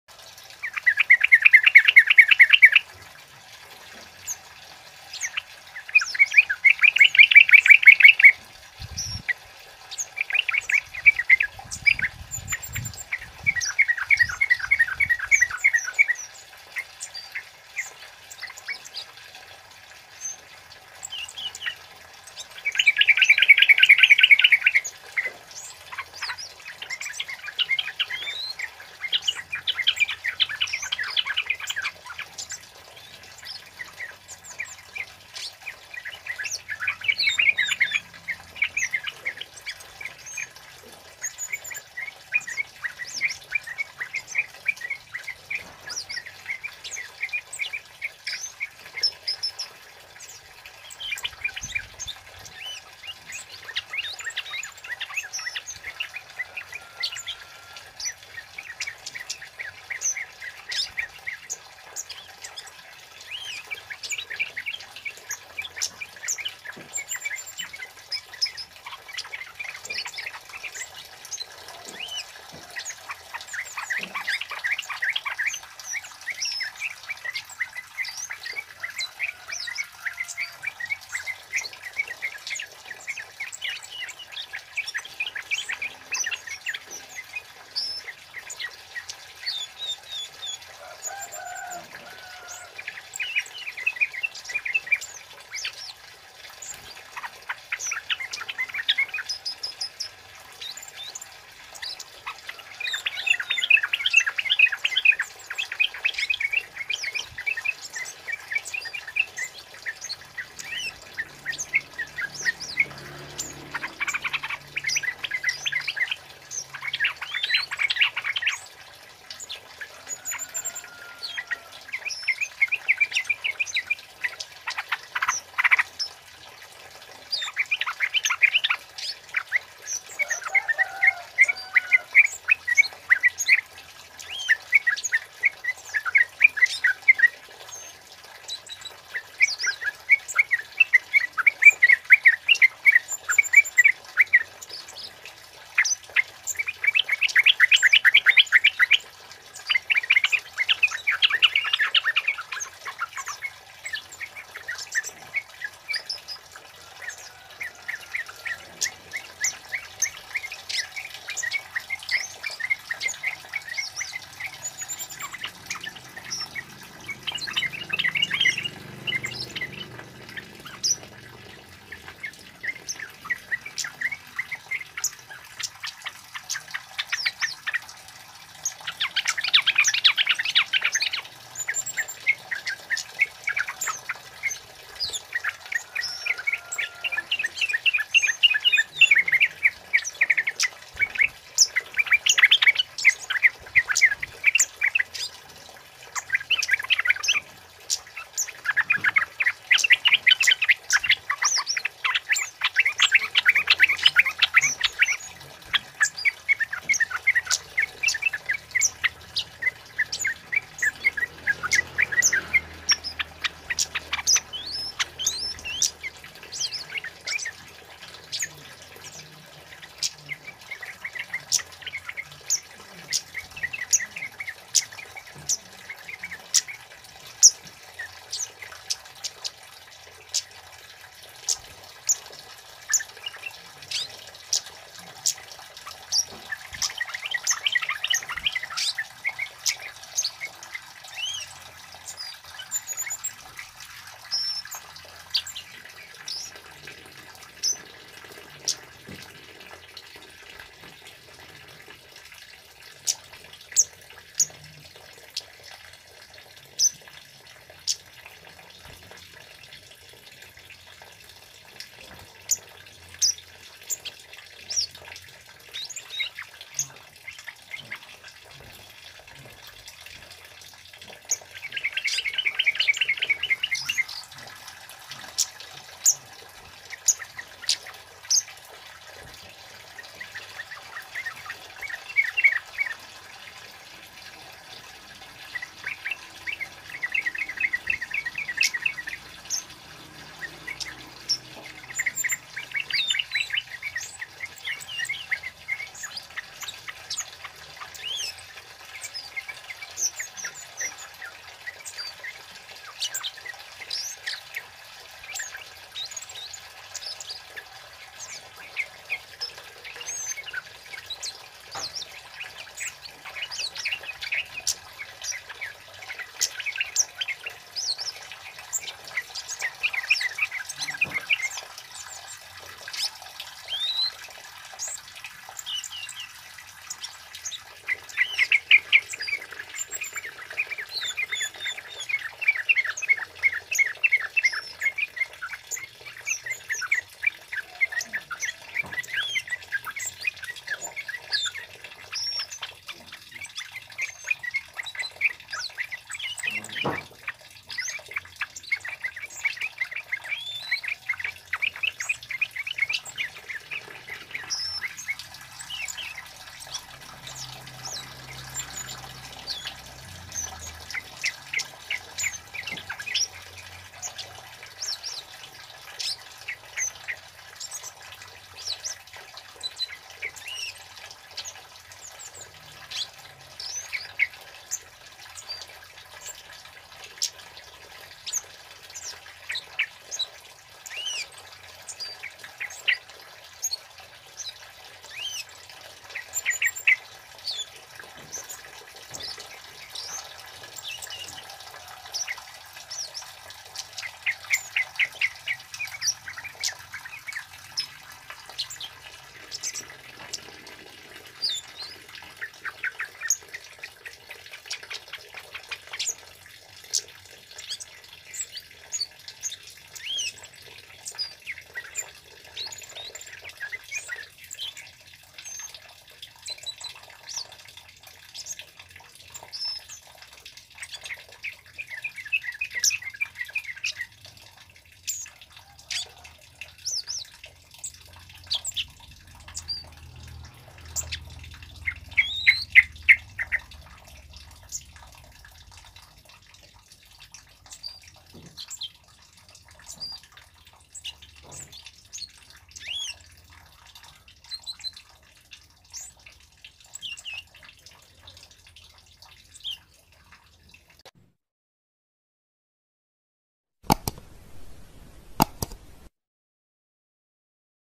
Suara Burung Kemade Cit Keres Gacor Jantan Buat Pikat Dan Masteran
Suara Burung Kemade Cit Keres Jantan Gacor Buat Masteran Dan Pikat

assalamualaikum kawan sahabat youtube kali ini aku video suara kemade gacor Ngropel ngeplong cocok buat pancingan dan masteran salam kicau mania mantap